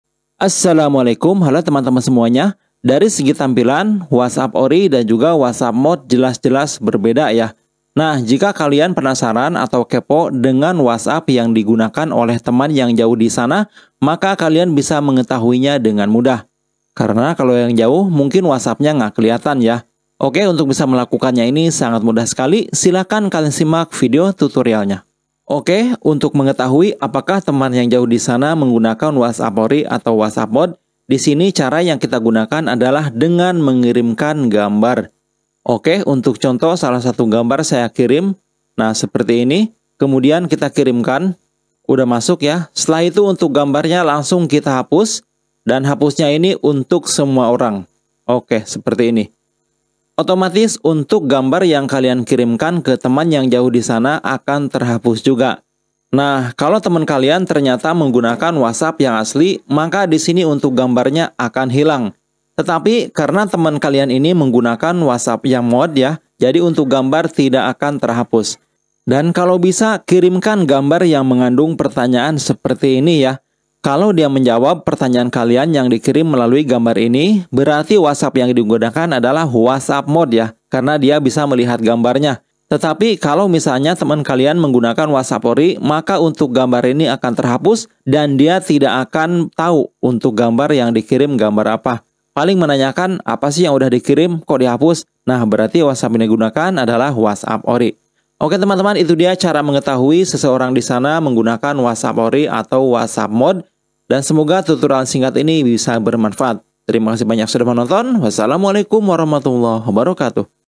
Teman Kamu Menggunakan WhatsApp ORI Atau Mod ? 🔥 Begini Cara Mengetahuinya
Telah sejak lama aplikasi WhatsApp yang dimodifikasi seperti GB WhatsApp,Aero,FM, dan yang lainnya, memiliki banyak fitur unggulan yang tidak dimiliki oleh WhatsApp yang versi orinya atau resmi, dan jika salah satu teman kalian menggunakan WhatsApp yang mod, maka tentu saja dia akan memiliki berbagai macam fitur yang tidak ada di WhatsApp kalian.

Nah jika Kalian ada yang penasaran dengan jenis aplikasi WhatsApp yang digunakan oleh teman,apakah dia menggunakan WhatsApp mod atau yang ORI, ada cara yang cukup mudah untuk mengetahuinya, silakan simak video tutorialnya ya.

Terimakasih